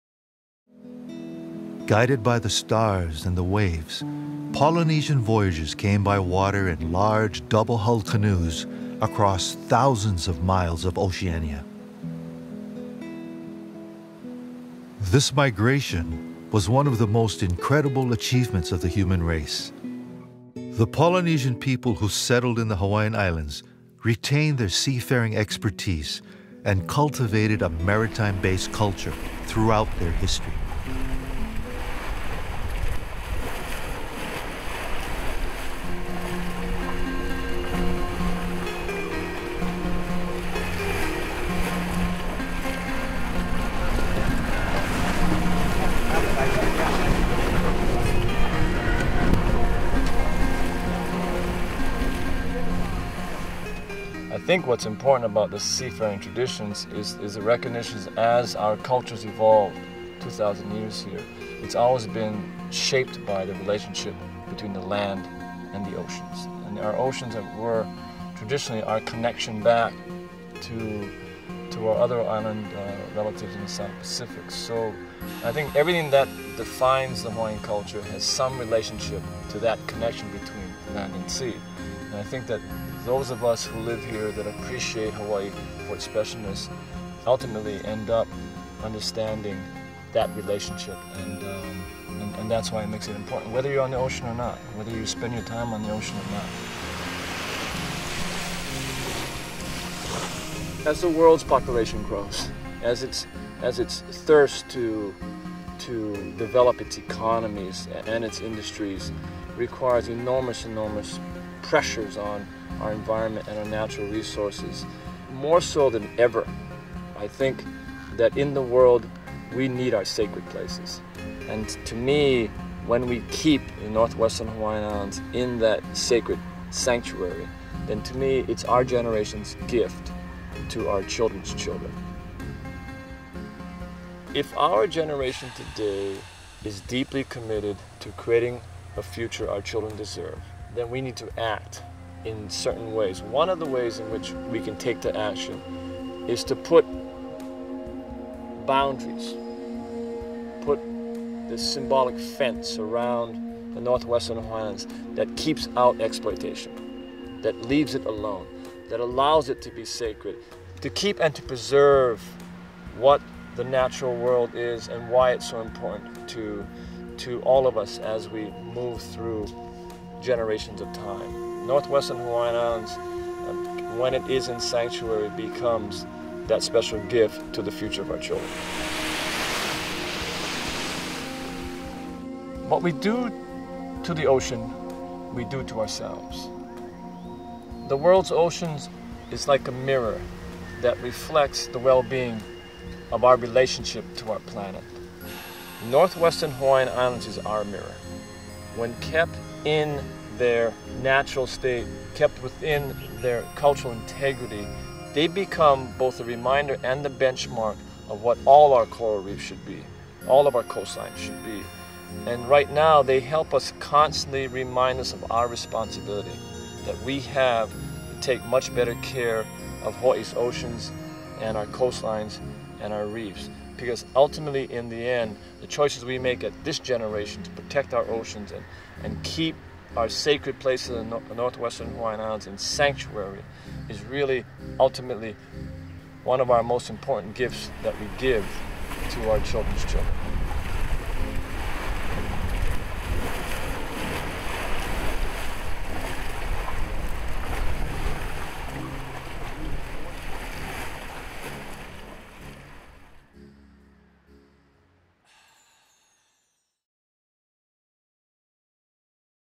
Marine Protected Areas - NWHI
Learn about the sacredness of the Northwestern Hawaiian Islands and the importance of marine protected areas.